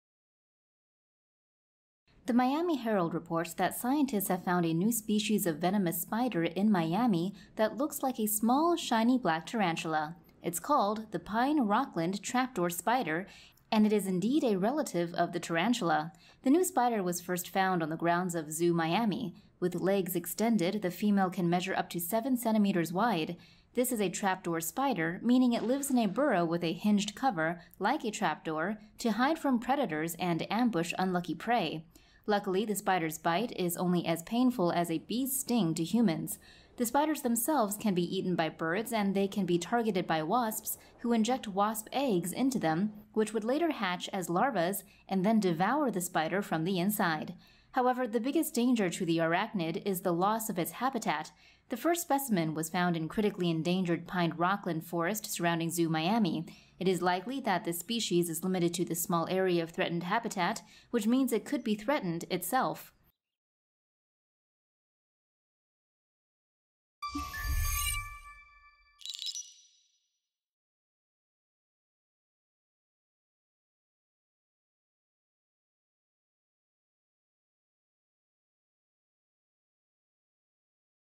Florida now home to venomous new tarantula species
Florida has finally found a new spider that is not an invasive species — but it's not for the squeamish.

RUNDOWN SHOWS:
1. Show new spider from many sides, then moving in its trapdoor home
2. Map zoom, from global to local, highlighting first Florida, then Miami zoo
3. Show scientist's tool as it is used to open trapdoor, spider jumps up to doorway
4. Show spider being eaten by bird, show wasp injecting eggs into spider's abdomen
5. Show spider being eaten by wasp larvae, many larvae coming out of spider's abdomen
6. Show aerial view of spider's small habitat around zoo, hemmed in by city blocks

VOICEOVER (in English):
The Miami Herald reports that scientists have found a new species of venomous spider in Miami that looks like a small, shiny black tarantula.

It's called the Pine Rockland Trapdoor Spider and it is indeed a relative of the tarantula.

The new spider was first found on the grounds of Zoo Miami. With legs extended, the female can measure up to seven centimeters wide.

This is a trapdoor spider — meaning it lives in a burrow with a hinged cover, like a trapdoor, to hide from predators and ambush unlucky prey.

Luckily, the spider's bite is only as painful as a bee's sting to humans.

The spiders themselves can be eaten by birds and they can be targeted by wasps, who inject wasp eggs into them, which would later hatch as larvas and then devour the spider from the inside.

However, the biggest danger to the arachnid is the loss of its habitat.

The first specimen was found in critically endangered pine rockland forest surrounding Zoo Miami.

It is likely that this species is limited to this small area of threatened habitat, which means it could be threatened itself.